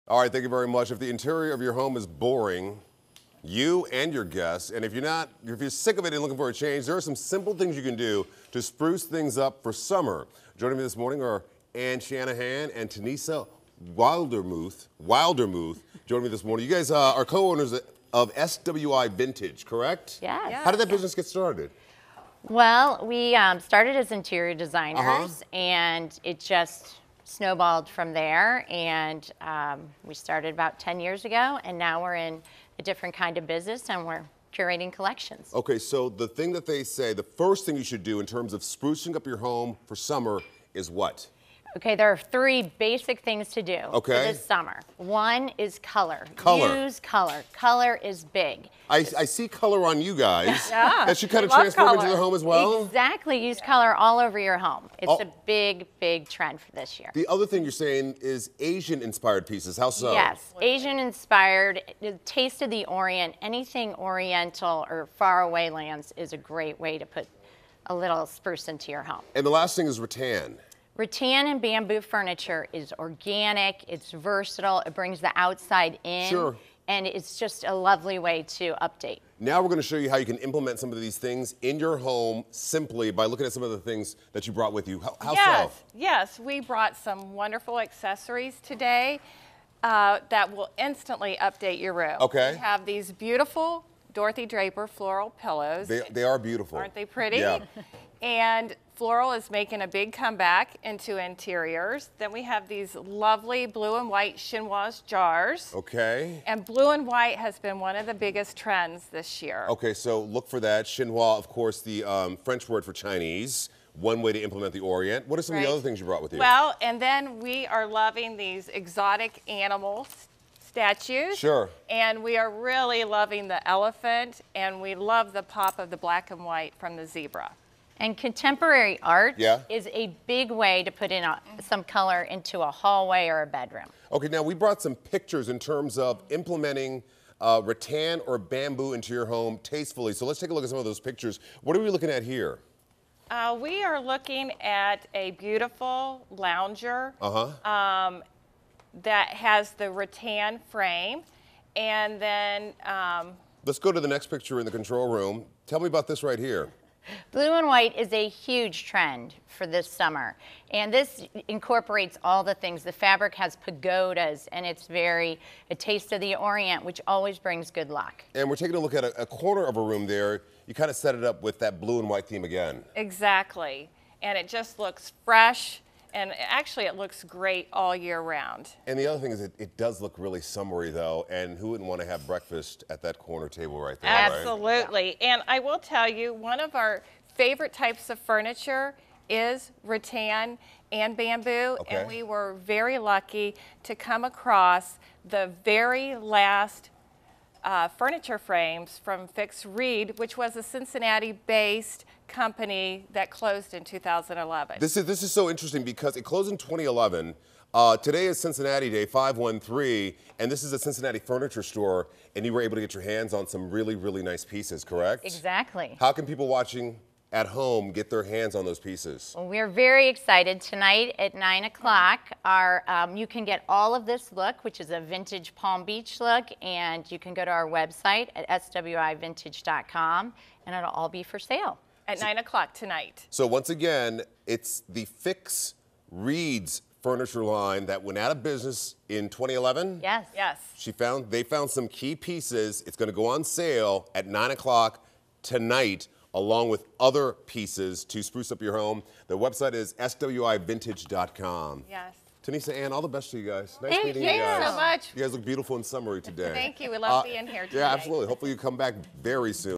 SWI Vintage Ficks Reed Furniture on FOX19
SWI Vintage explains the easy ways to implement this year's interior design trends into home and unveils their exclusive Ficks Reed furniture sale.